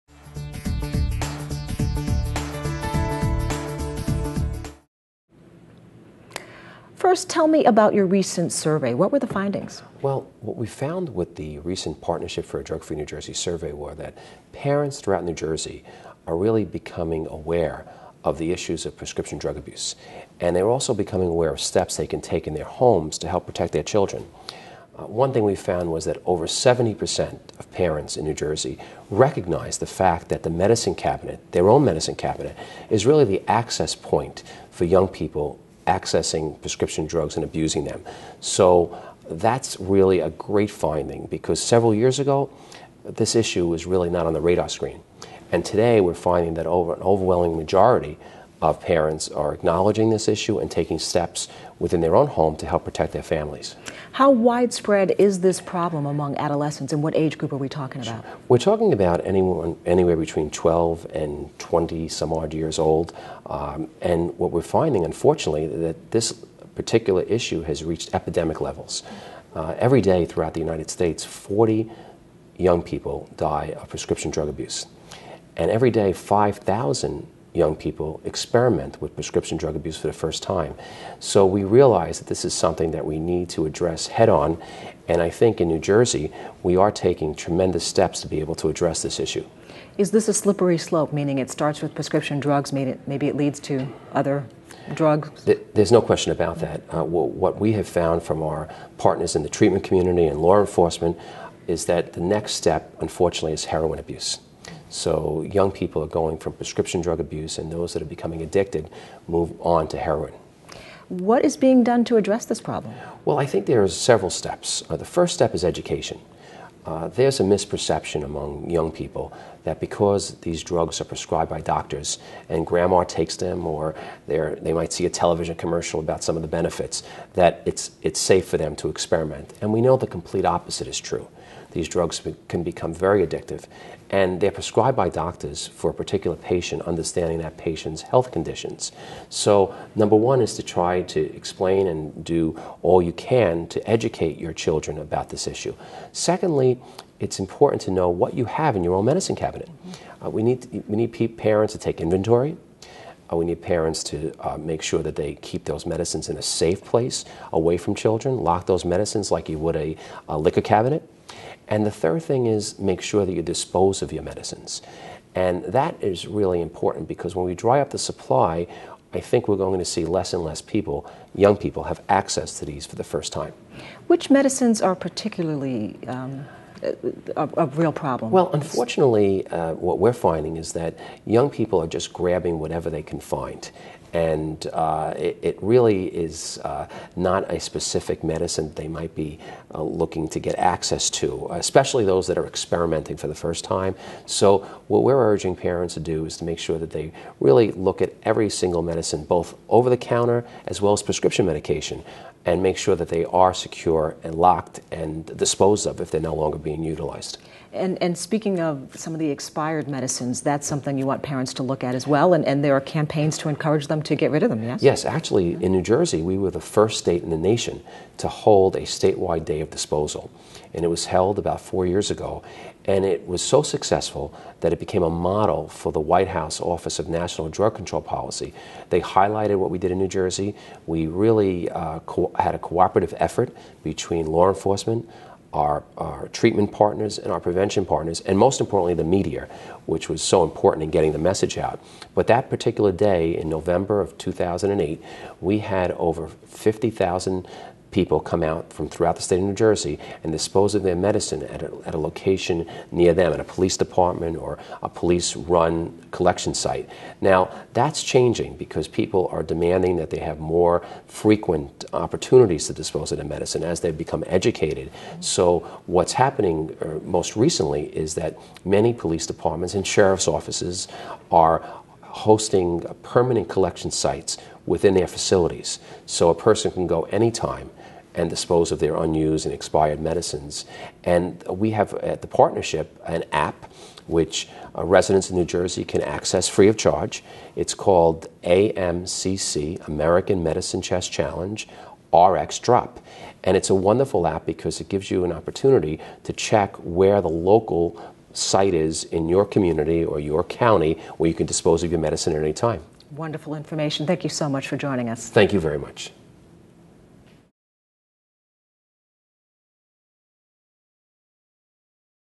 Abuse of Prescription Drugs by Young People at Epidemic Levels
The Partnership for a Drug-Free New Jersey (PDFNJ) recently released the results of a statewide survey that shows about eight in 10 parents believe kids get prescription and over-the-counter drugs that they abuse from medicine cabinets in their homes or their friends' homes. PDFNJ Executive Director Angelo Valente told NJ Today Senior Correspondent Desiree Taylor that the survey shows parents are beginning to recognize the issue of prescription drug abuse and have begun taking steps to prevent it. He also said educating young people about the dangers of prescription drugs is key to curtailing abuse. For more New Jersey news, visit NJ Today